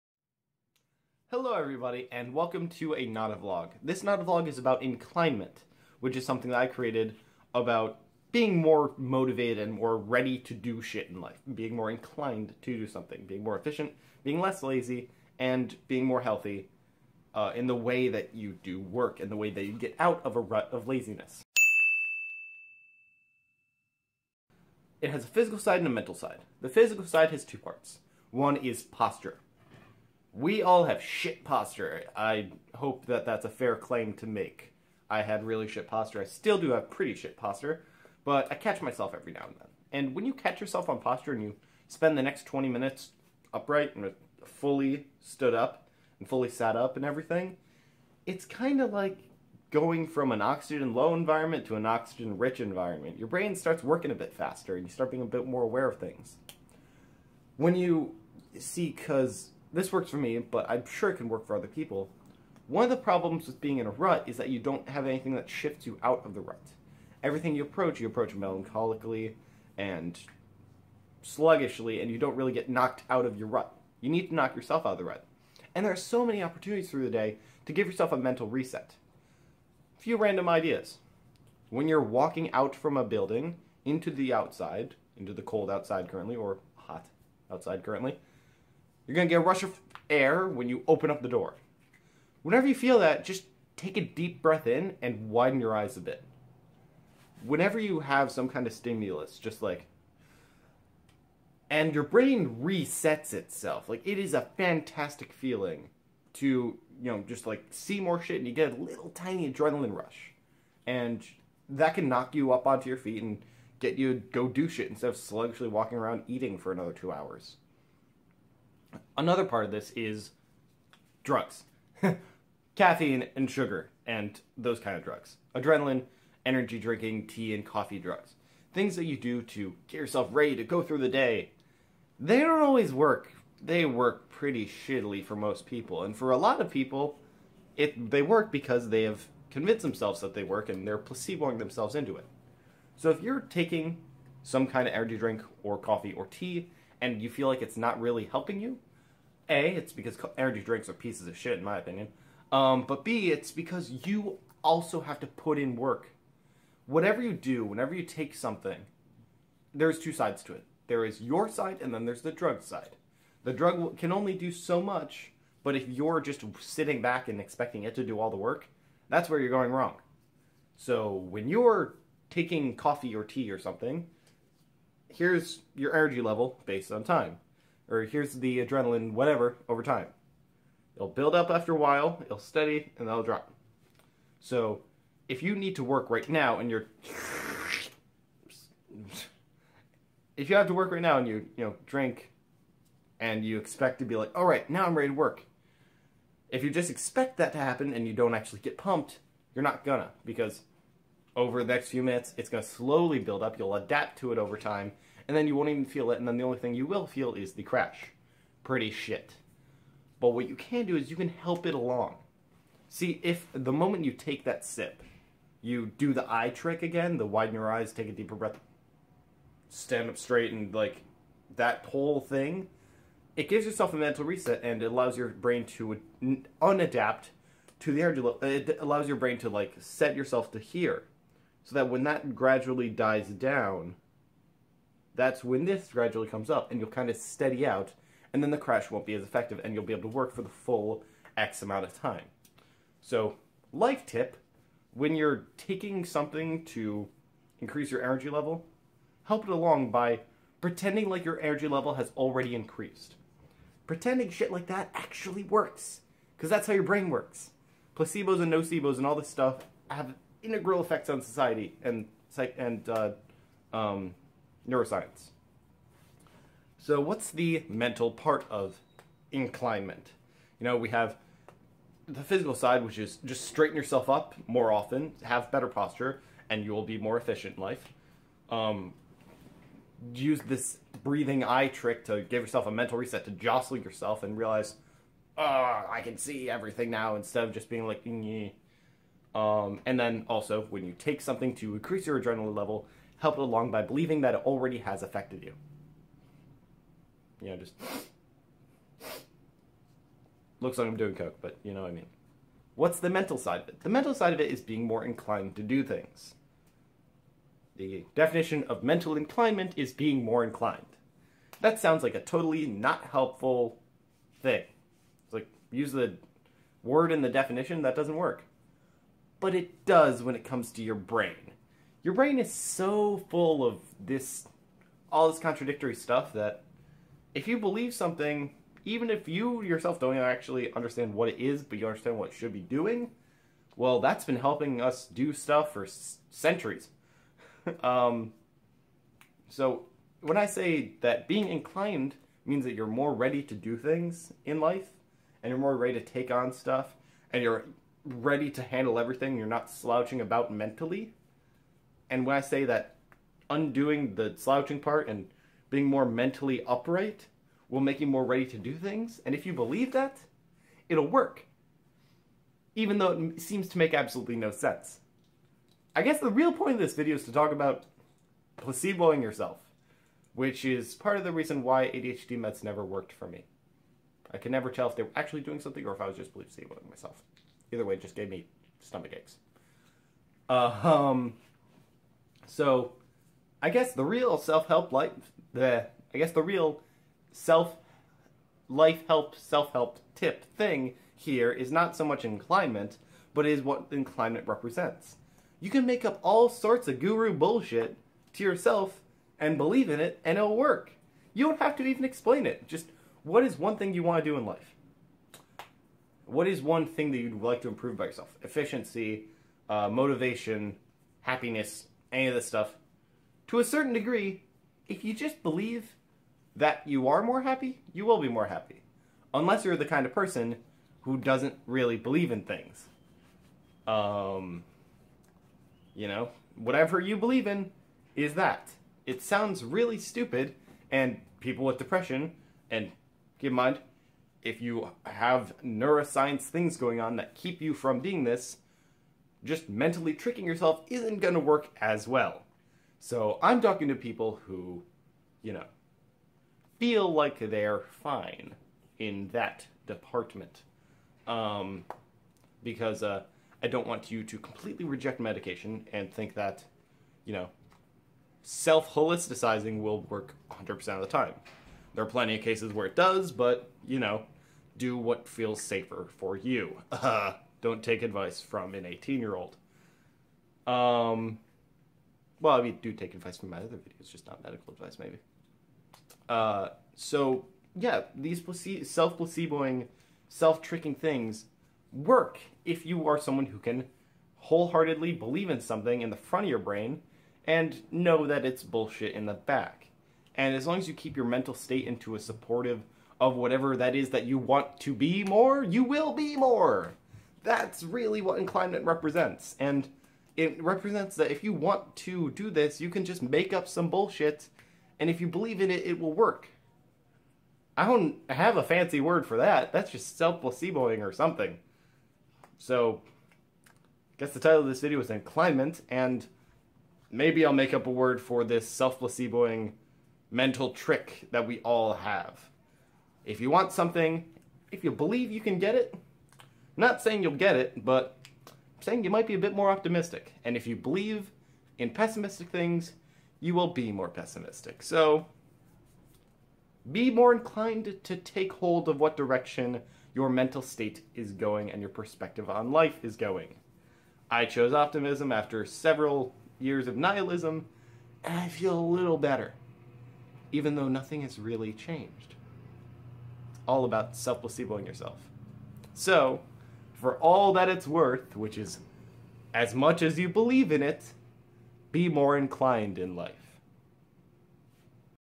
Inclinement and Self Placeabos!
Inclinement is the ideology of self-help through being more inclined in life! . . . Something doesn't seem right here.

Please check out all of The T_P_T Network on my Channels Page!